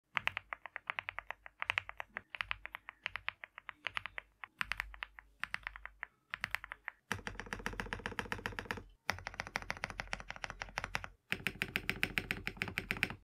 Meletrix Zoom98 ASMR
Keeb-rolling with @meletrix2022 Zoom98 (dot proto).

Got out of my usual colorway and went for the milky green this time. Quite worried on how to match this kit but luckily and goes super will with @switchlab_ Maclabs Keycaps. I love it together, I’m 🫠.

Build :
Keyboard : Meletrix Zoom98 in Milky Green
Switch : MZ Studio Spring Linear Switch
Keycaps : Maclabs keycaps by Switchlabs